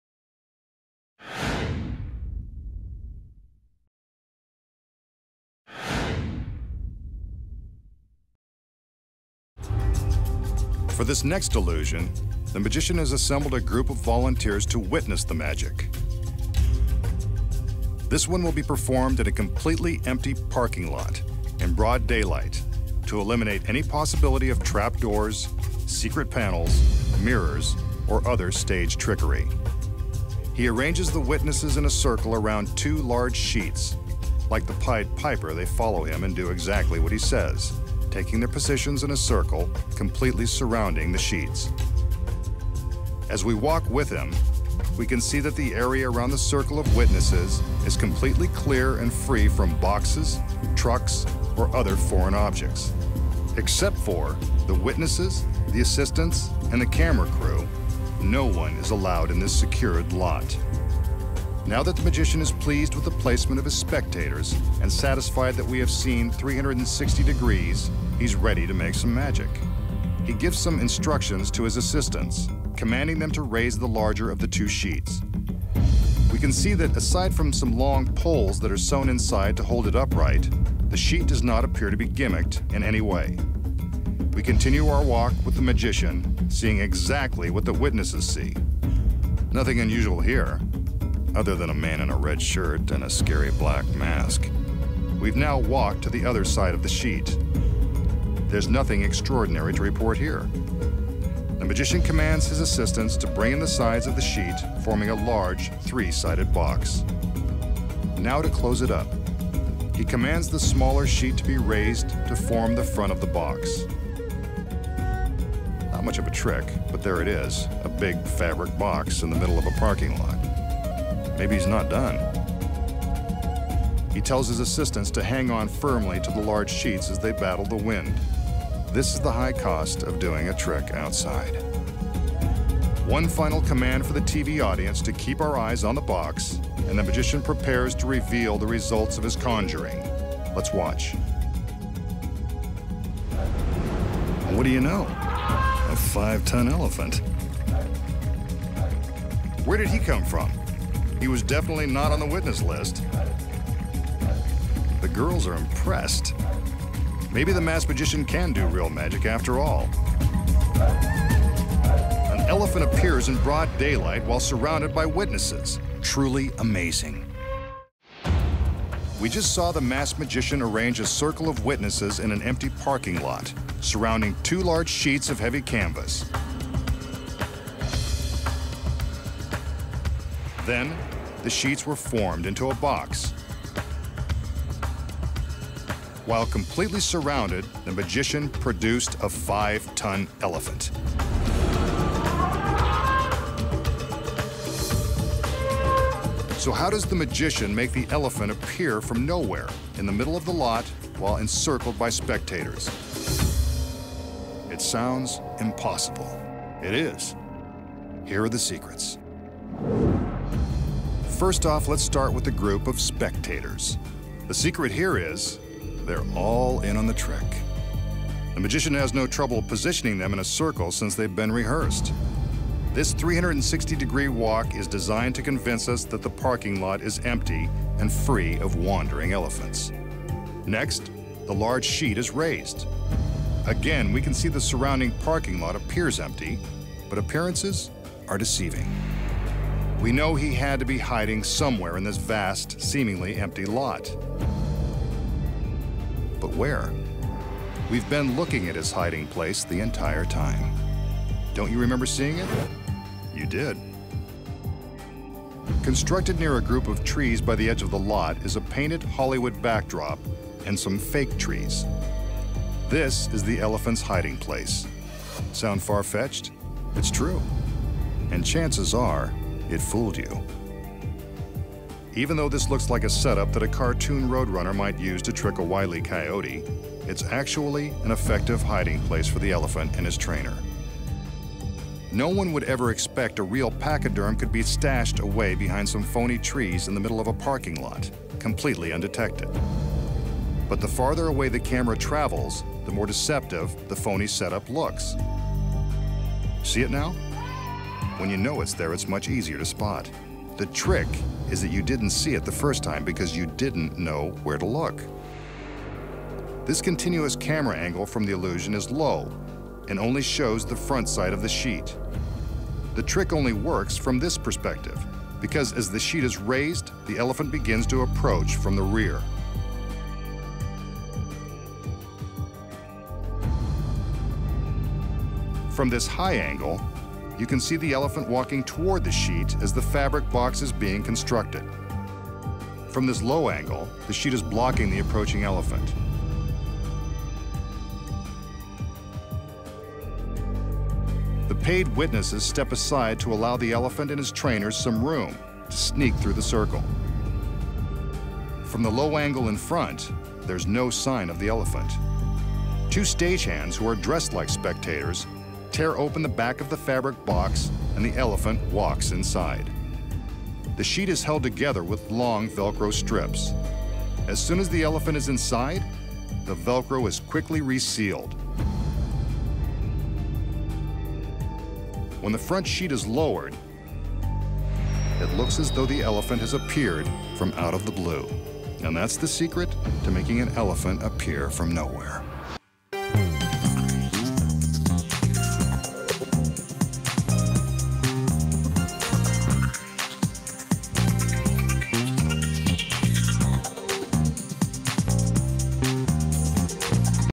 FIVE-TON ELEPHANT APPEARS OUT OF THIN AIR!
First it's not there. then it is. How does our magician wave his hands and make a five-ton pachyderm suddenly appear out of nowhere? Watch the video and learn the incredible secret of how he does it!